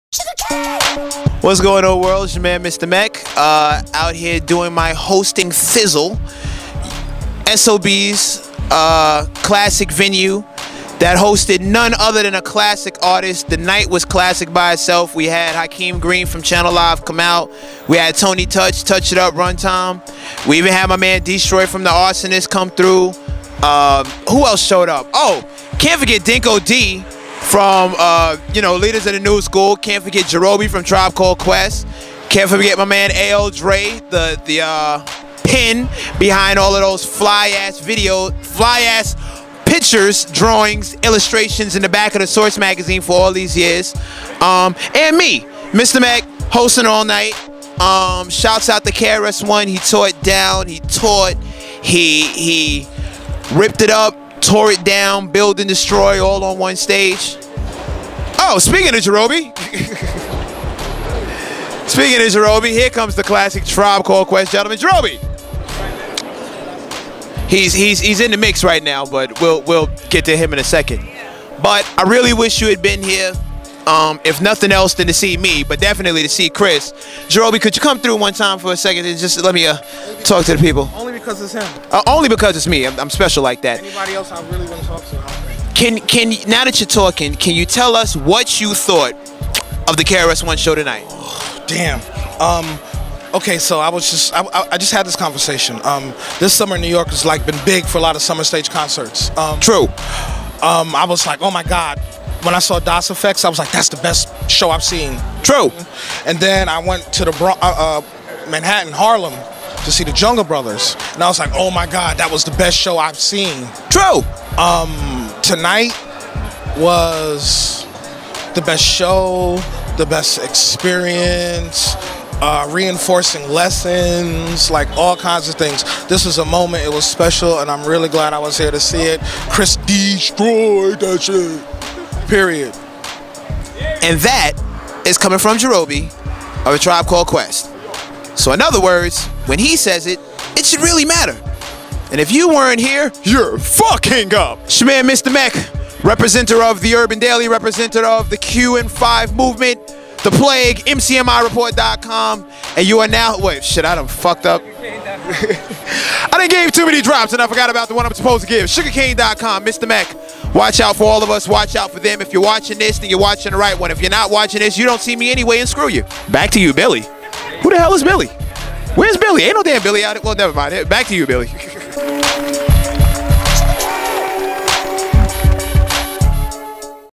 Mr Mecc And Jarobi Talk About The KRS One Night Only Show At SOBs
I caught up with Mr Mecc after he hosted the amazing KRS-ONE "One Night Only" show at SOBs. During the interview Mr Mecc got Jarobi from A Tribe Called Quest to tell what he thought about the show. We all loved the show, it was HipHop Dopeness at it's highest level! Like I said in my other post about the KRS-ONE show, If you call yourself a HipHop head and wasn't in the building for the KRS Show SHAME ON YOU!! It was hands down the beast HipHop show of the year!